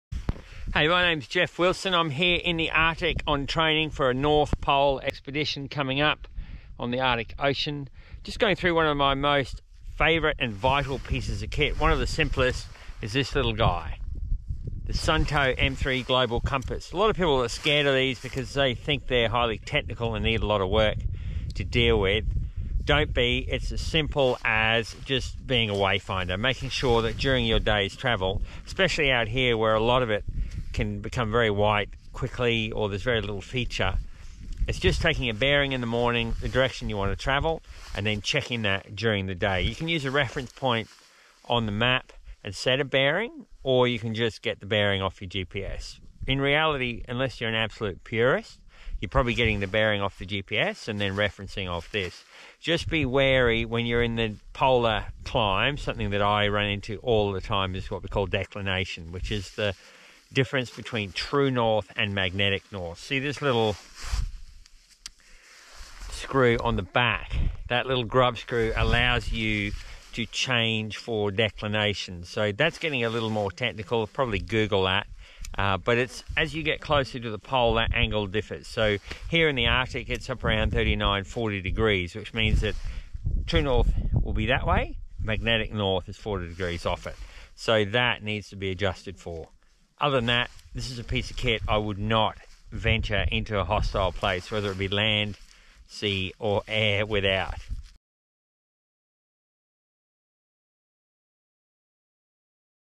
SUUNTO M3 GLOBAL COMPASS: HOW TO USE A COMPASS
Polar Explorer Geoff Wilson shares one of his favourite pieces of kit from Suunto during a polar expedition test run in the arctic. 🧭